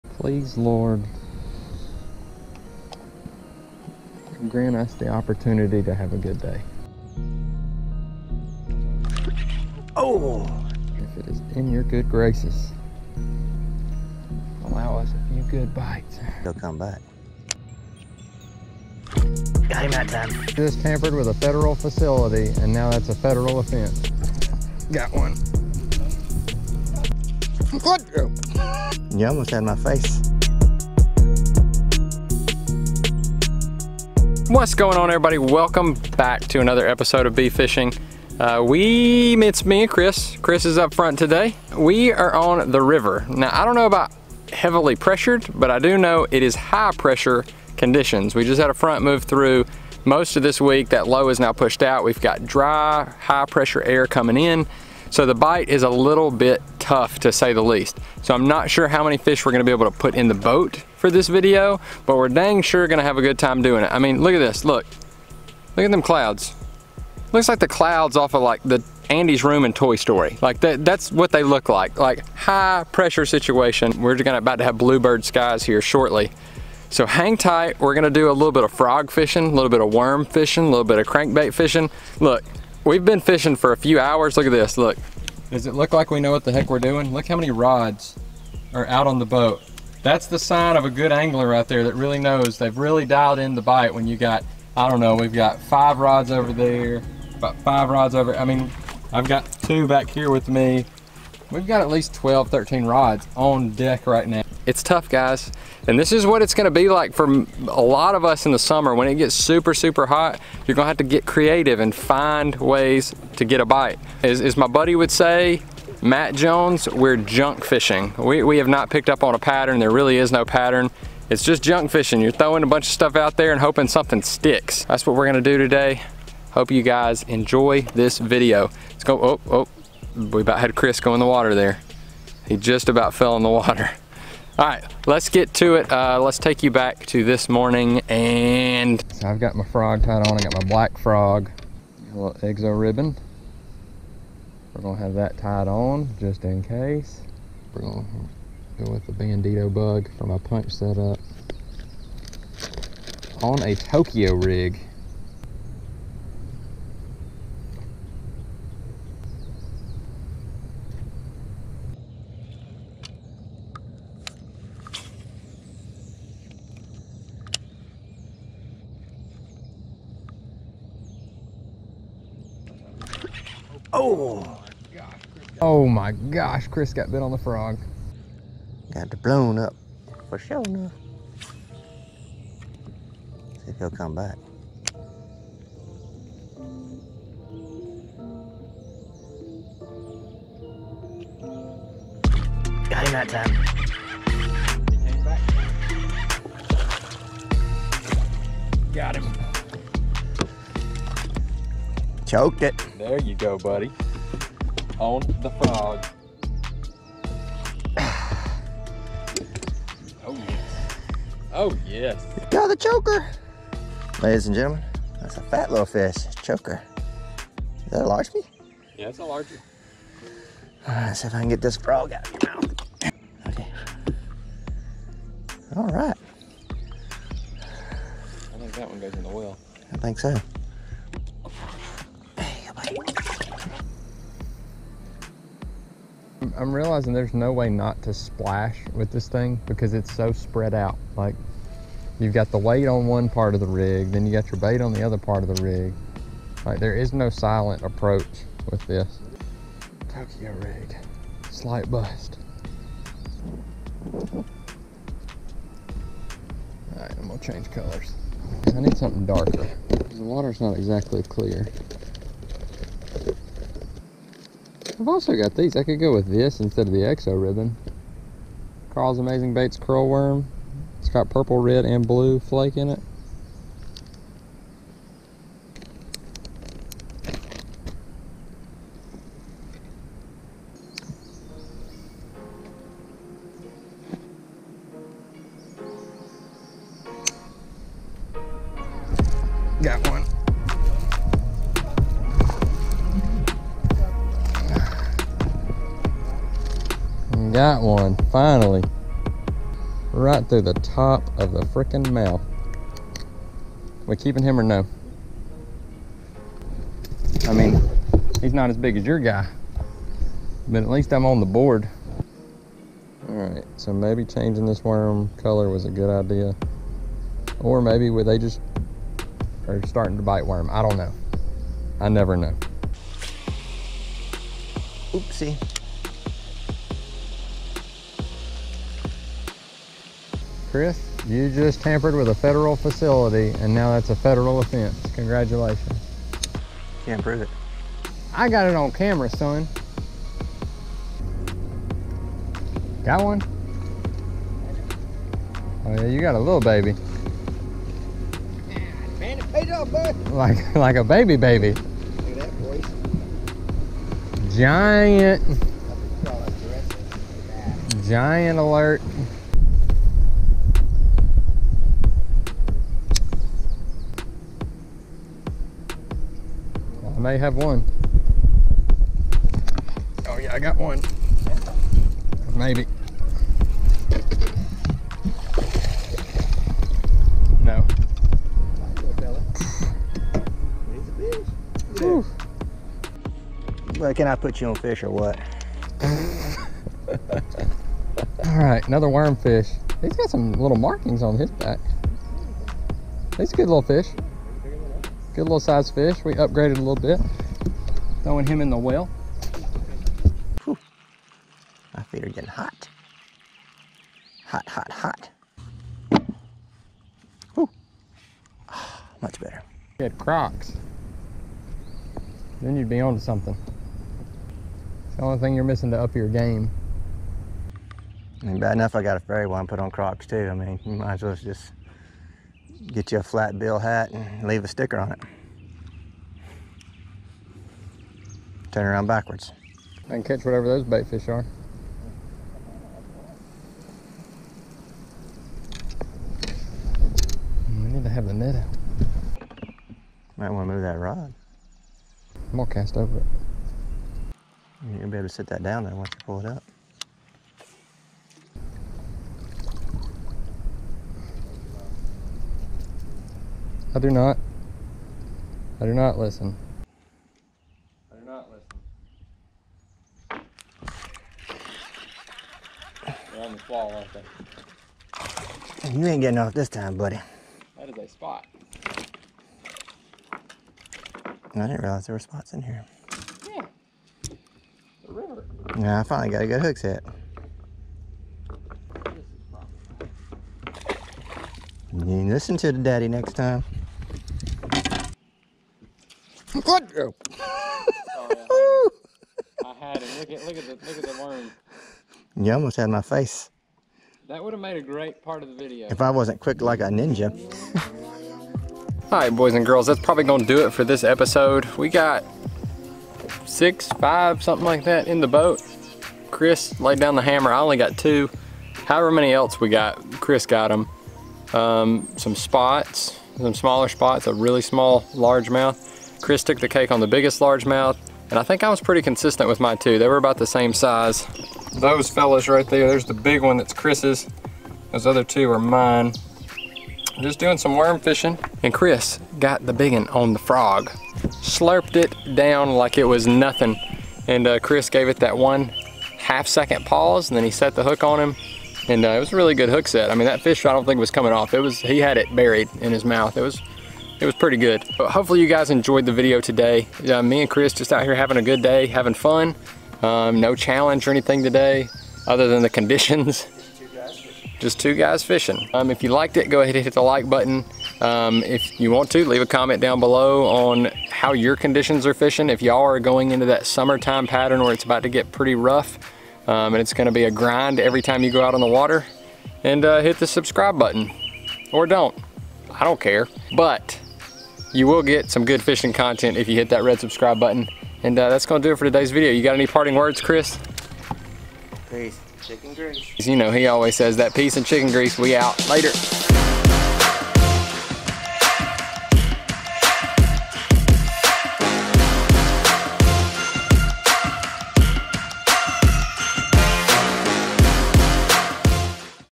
Bass DESTROYED This FROG!!! (EPIC CATCH)
We hit up the river and the conditions were less than ideal. High pressure and heavily pressured, if you know what I mean. The plan was to do some frog fishing and flipping but sometimes you have to throw everything at them until something works.

Use code: BFISHING
(Works for first MTB)